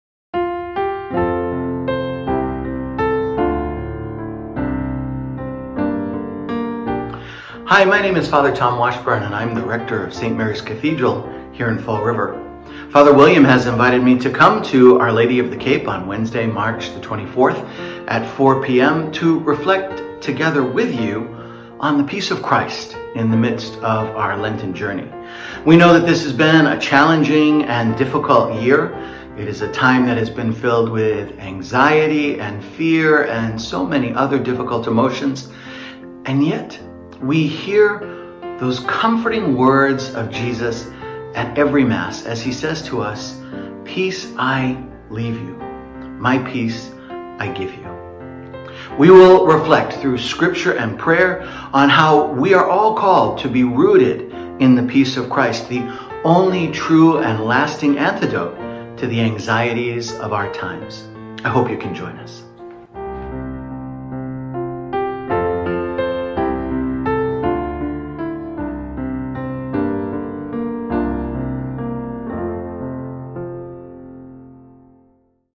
Parish Lenten Mission | Our Lady of the Cape | March 24, 2021
Parish Lenten Mission
March 24, 2021 at 4pm
Our Lady of the Cape Church
Brewster, Massachusetts

Held both in church and online right here on YouTube and our parish